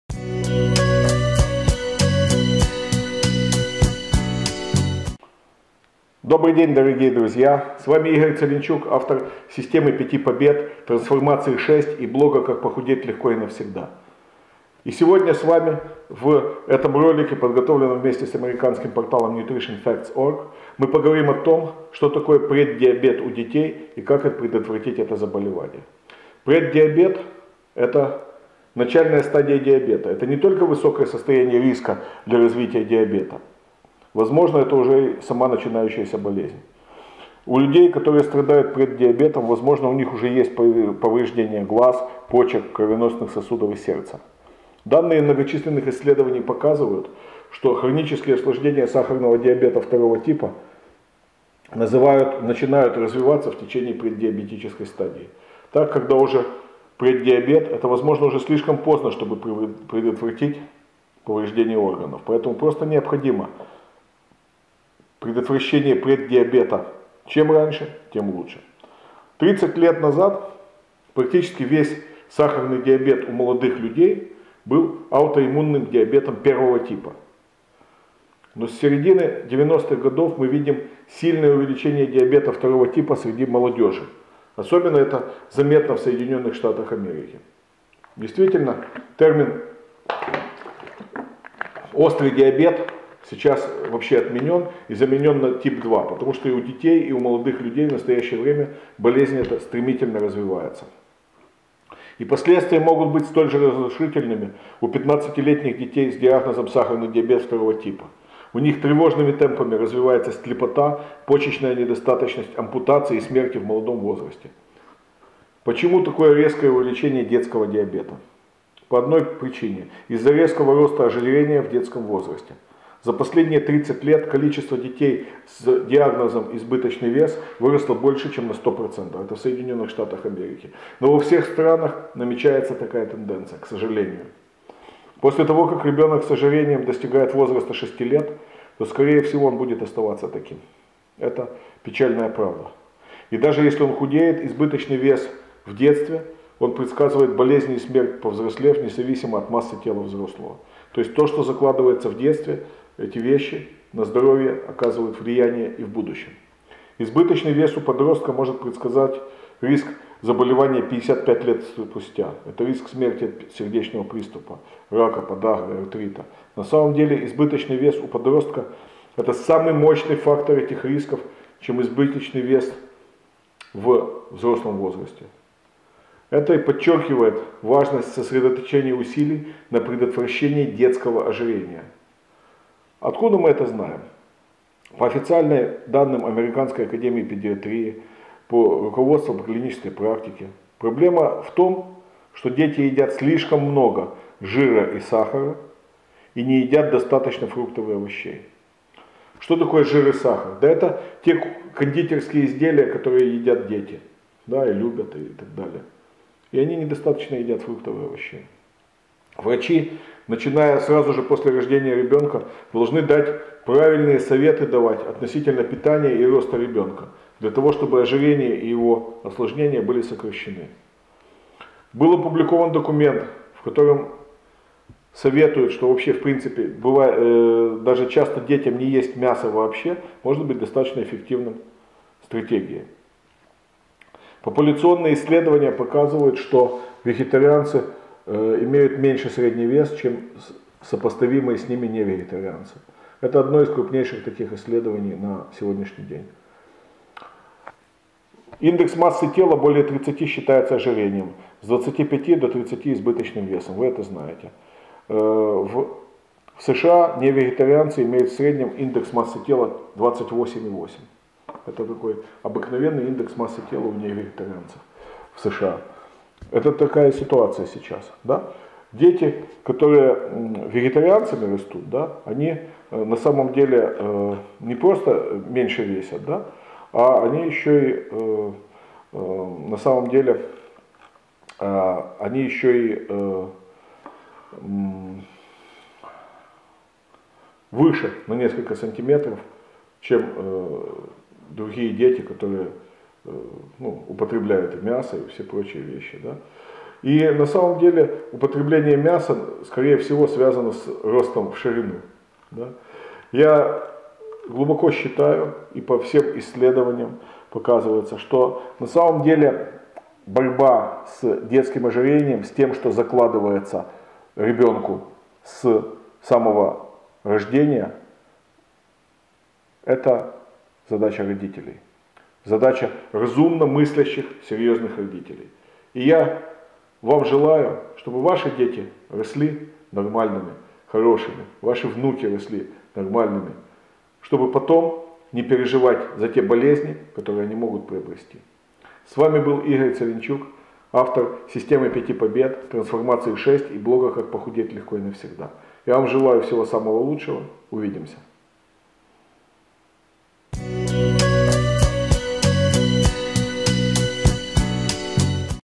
Преддиабет у детей, как предотвратить заболевание
Бесплатный курс легкого снижения веса.

Преддиабет не только высокое состояние риска для развития диабета – преддиабет, может быть, уже сама болезнь. У людей, которые страдают преддиабетом, возможно, у них уже есть повреждение глаз, почек, кровеносных сосудов, и сердца.
Данные многочисленных исследований показывают, что хронические осложнения сахарного диабета 2 типа, начинают развиваться в течение преддиабетической стадии. Так, когда у нас уже преддиабет, это, возможно, уже слишком поздно, чтобы предотвратить повреждение органов, поэтому просто необходимо предотвращение преддиабета и чем раньше, тем лучше.
Тридцать лет назад, практически весь сахарный диабет у молодых людей был аутоиммунным диабетом 1-го типа, но с середины 90-х годов мы видим увеличение диабета 2-го типа среди нашей молодежи, особенно в Соединенных Штатах. Действительно, термин острый диабет в настоящее время отменен и заменен на "Тип 2", потому что у детей, и у молодых людей, в настоящее время, болезнь стремительно развивается.
И последствия могут быть столь же разрушительными, у пятнадцатилетних детей, с диагнозом сахарный диабет 2 типа, тревожными темпами развивается слепота, почечная недостаточность, ампутации и смерти в молодом возрасте.
Почему такое резкое увеличение детского диабета? Из-за резкого роста ожирения в детском возрасте. За последние 30 лет, количество детей, с диагнозом избыточный вес, выросло более чем на 100%.
Избыточный вес у подростка, может предсказать риск заболевания 55 лет спустя, это риск смерти от сердечного приступа, рака, подагры, артрита. На самом деле, избыточный вес у подростка, является более мощным фактором этих рисков, чем избыточный вес уже во взрослом возрасте. Это и подчеркивает важность сосредоточения усилий на предотвращение детского ожирения!